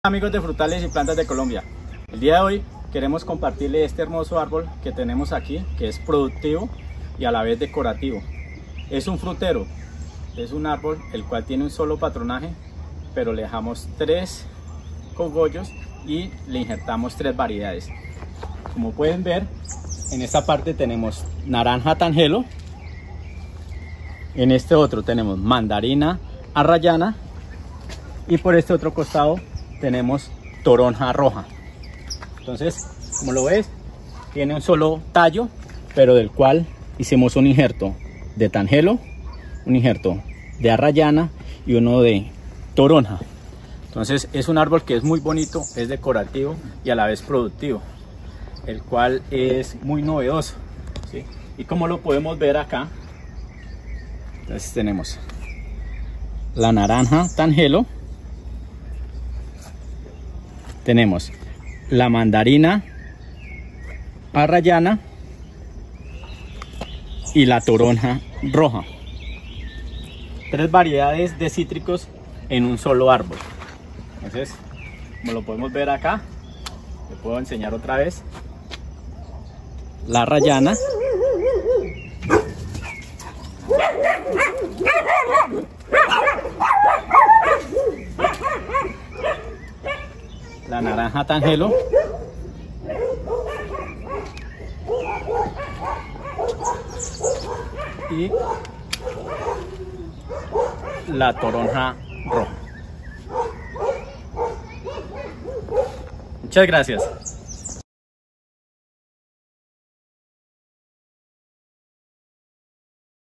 3 variedades en 1 solo árbol ( Naranja, Mandarina y Toronja)
Presentamos nuestro frutero, árbol injerto con 3 variedades diferentes ( Naranja Tangelo; Mandarina Arrayana; Toronja roja)